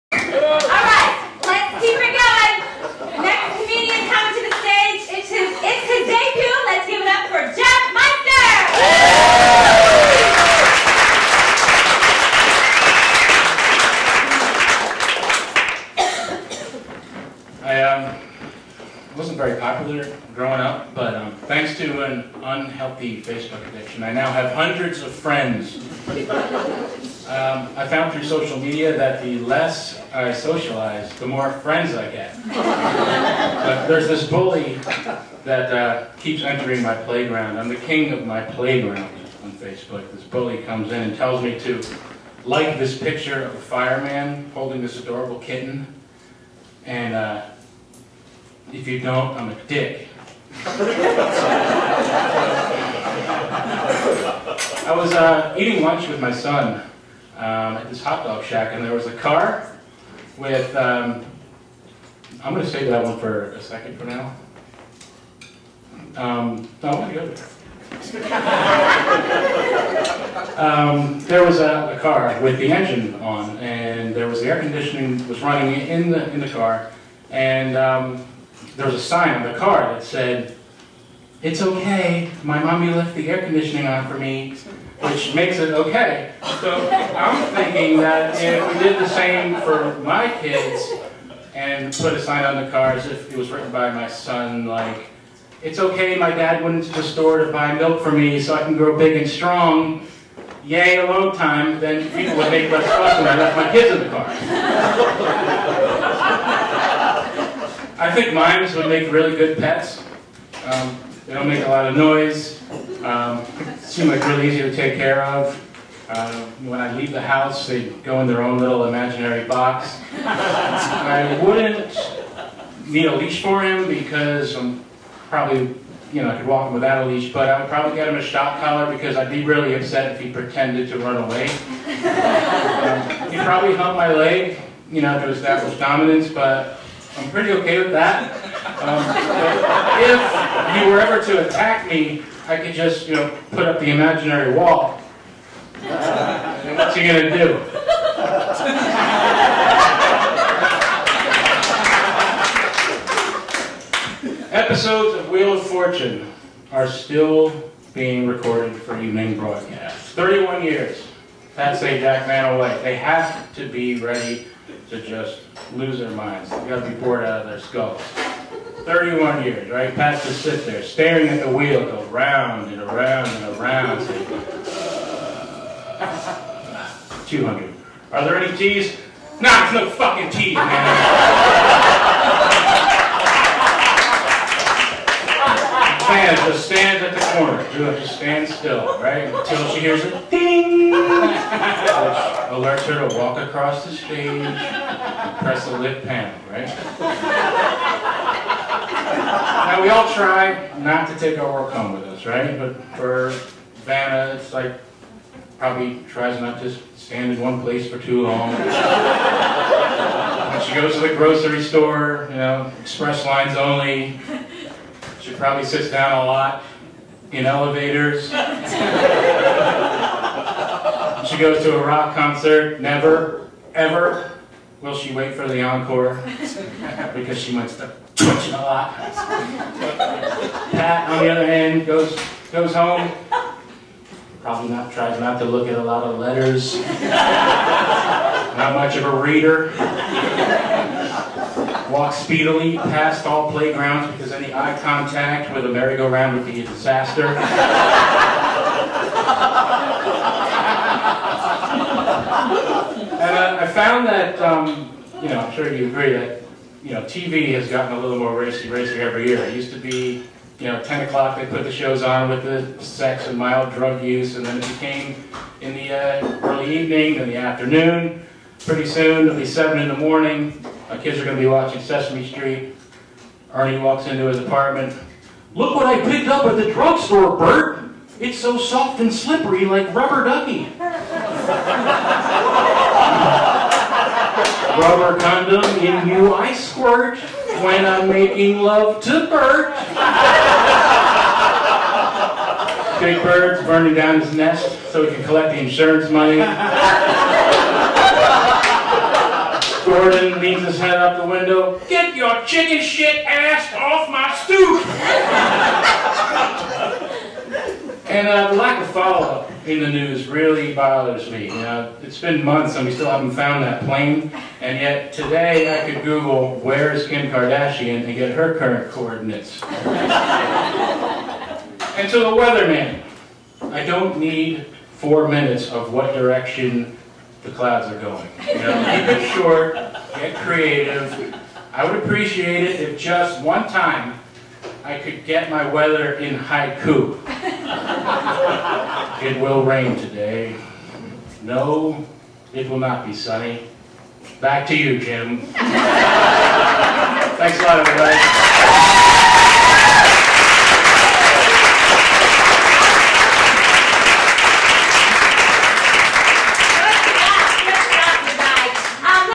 Jeff Meister at Gotham Comedy Club August 23, 2014
Jeff Meister performs with American Comedy Institute at Gotham Comedy Club August 23, 2014. For more information on American Comedy Institute's standup comedy classes,